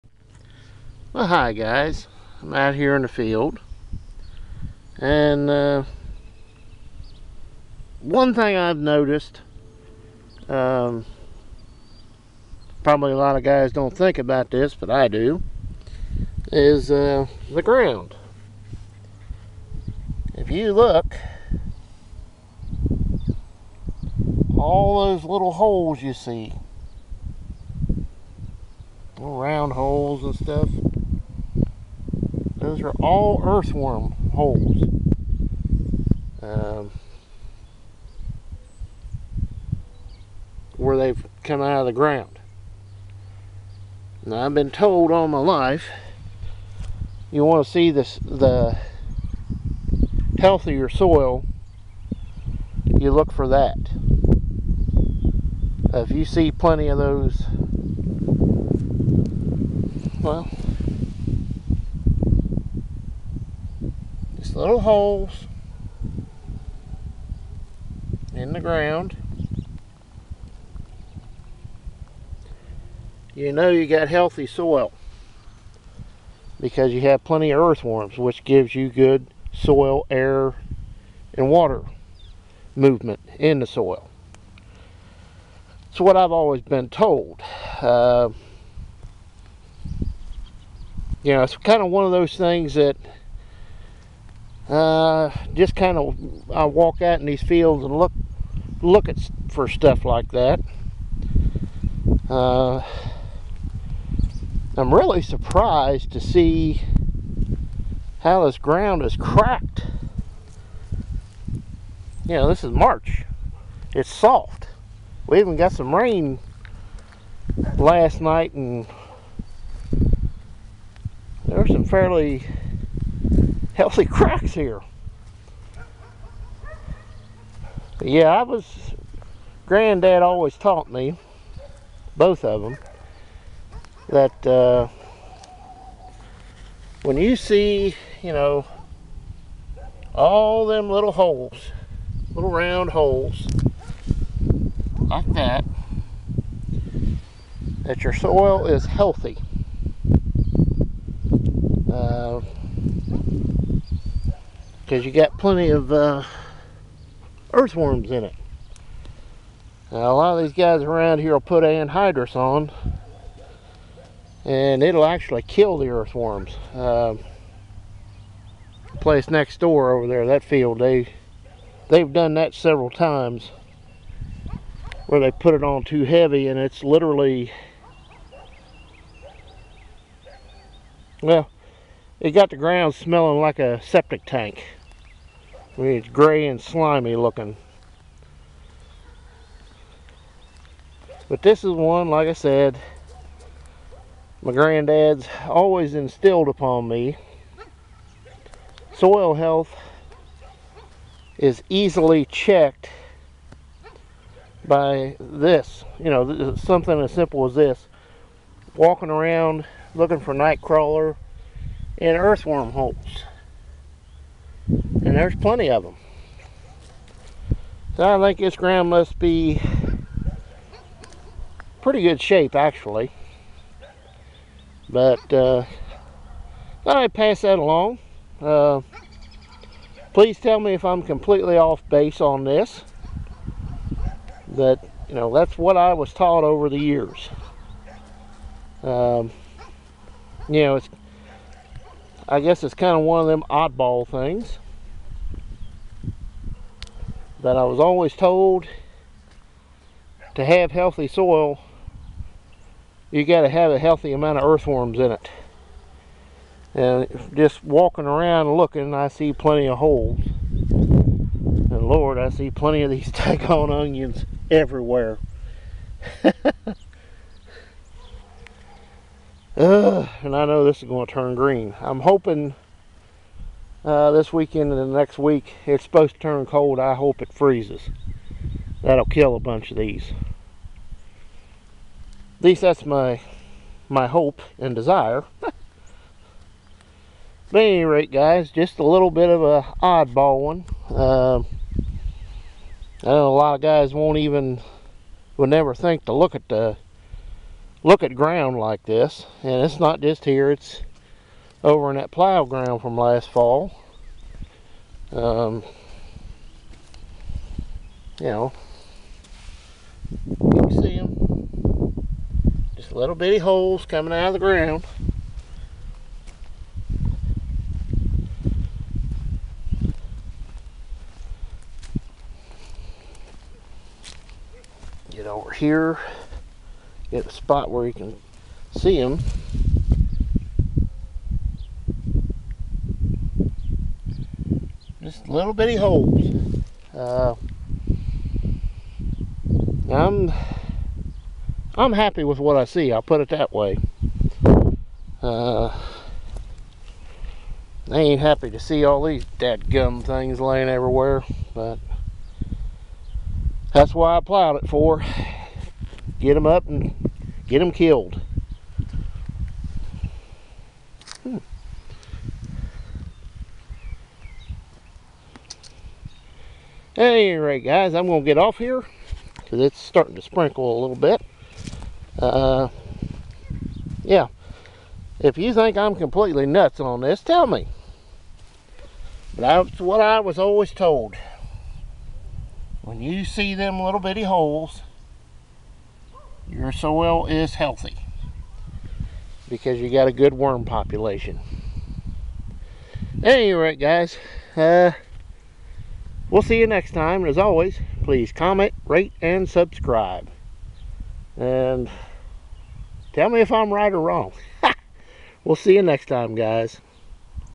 Checking Soil Health
After a great week and now getting some rain I went for a walk to see what the ground looks like. My grand fathers tough me that an easy way to see the health of the soil was to look for worm holes in the ground, The more you see the better.  Well walking around I see plenty of holes from the worms and night crawlers.  The ground looks good to me and has a good smell to it and lot of worm holes so I think its in good shape.  If I'm wrong  let me know I'm just going by what they told me all those years ago.  I hope you enjoy.  Bandit